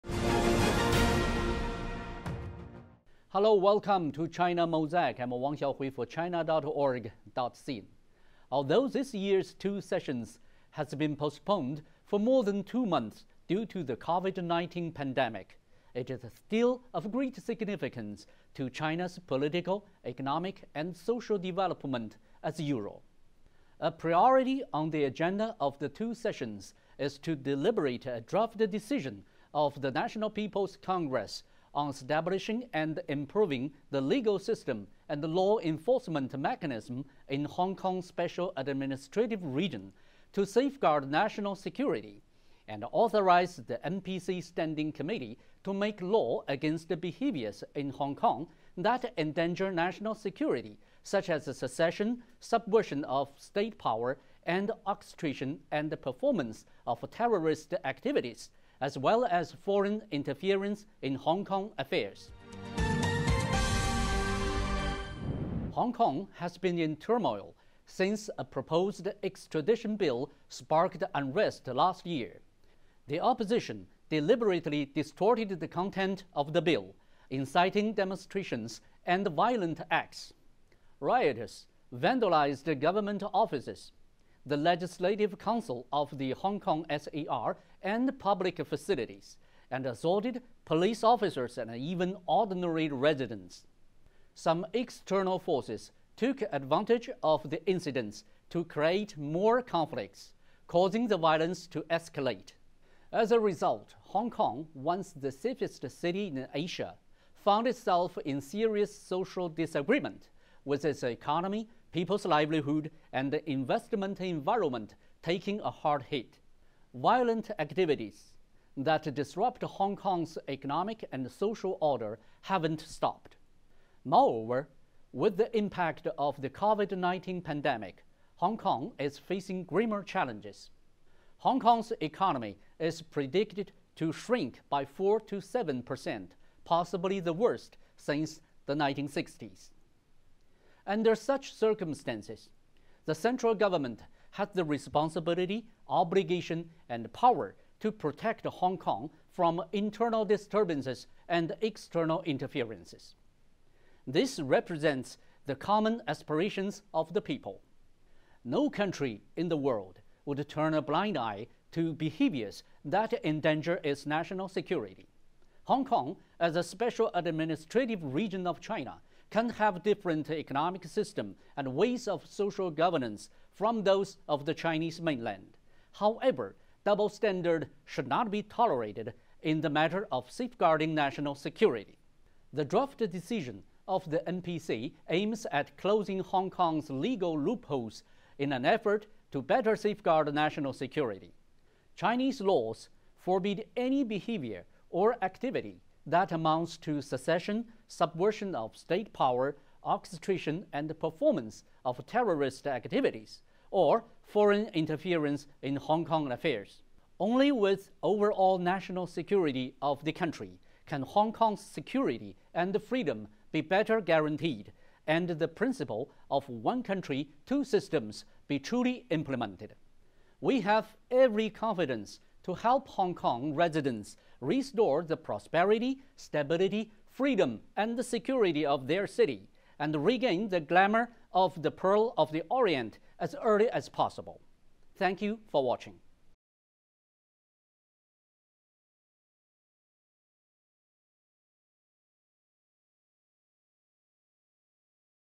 Hong Kong: No double standard in safeguarding national security
A priority on the agenda of this year's "twosessions" is to deliberate a draft decision of the National People's Congress (NPC) on establishing and improving the legal system and enforcement mechanisms for the HongKong Special Administrative Region (HKSAR) to safeguard national security.